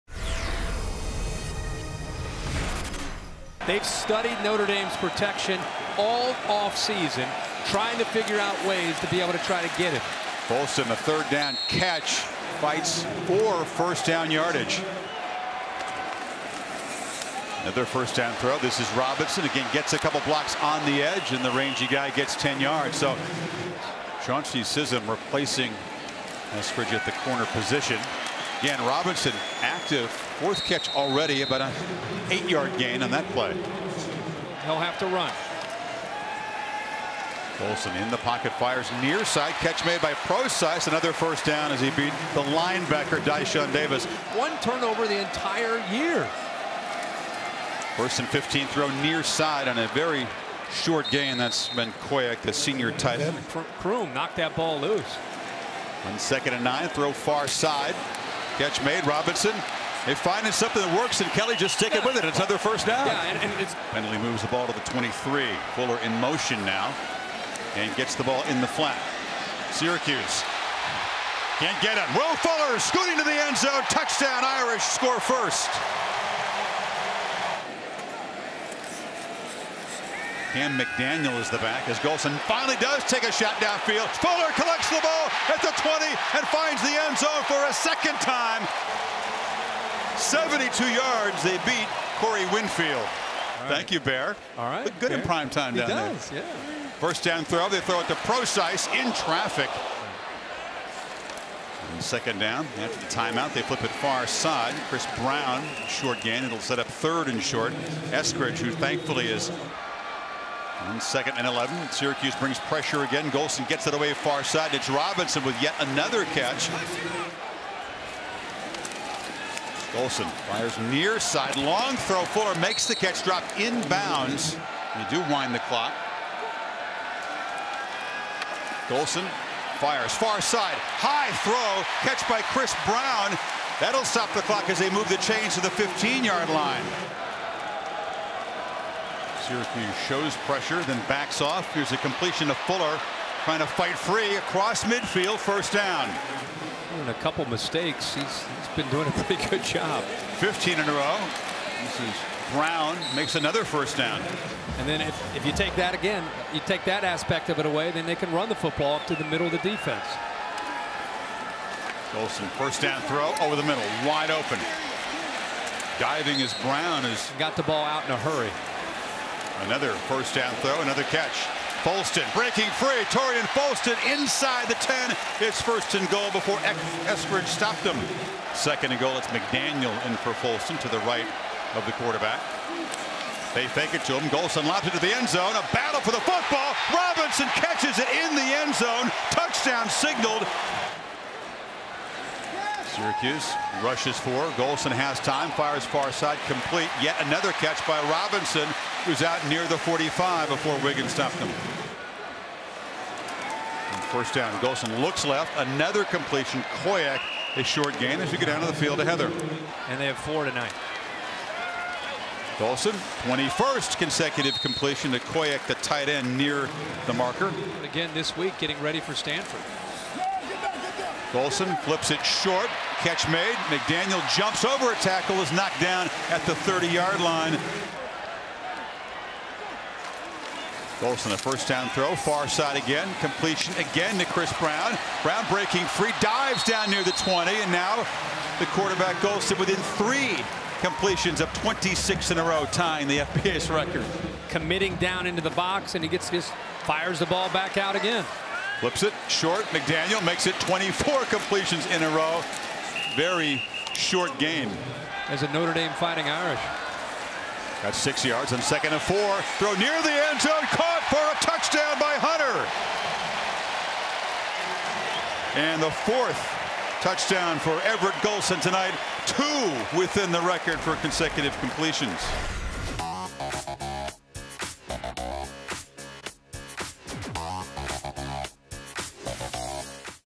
Notre Dame's Everett Golson Completes 25 Straight Passes
Notre Dame QB Everett Golson threw 25 straight completions in a win over Syracuse. Golson fell one completion short of the FBS record of 26 straight in the 31-15 victory. Watch as the ACC Digital Network shows you every completion during the streak.